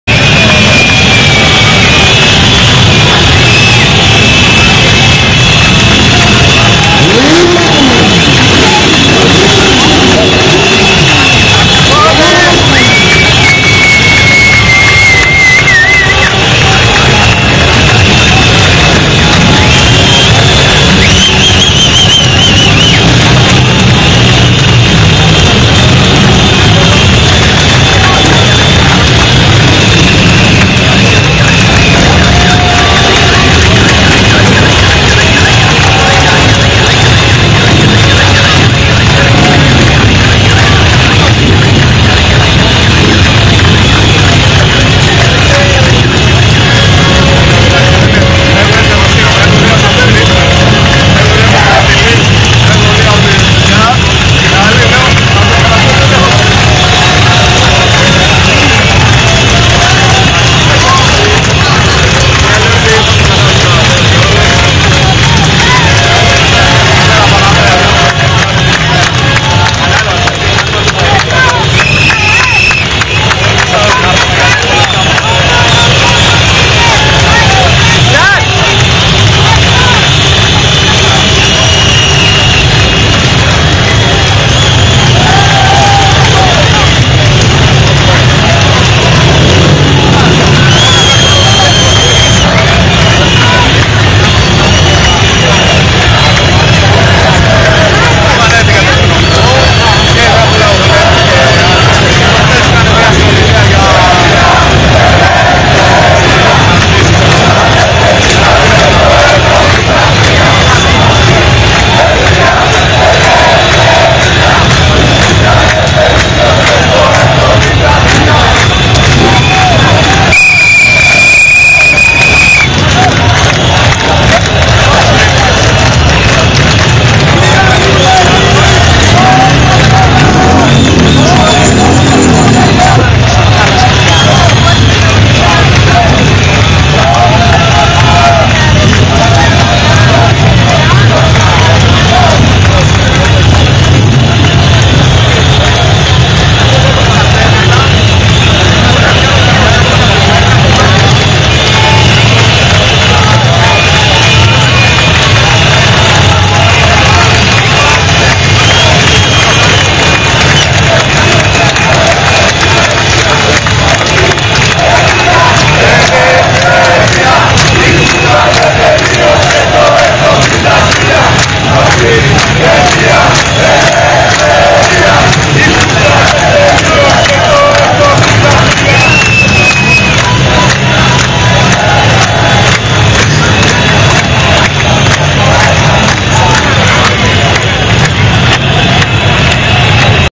Μηχανοκίνητη πορεία Σύνταγμα 28jgr
Μηχανοκίνητη πορεία φτάνει στην κάτω μεριά της πλατείας Συντάγματος. 28 Ιουνίου 2011, ώρα 19:00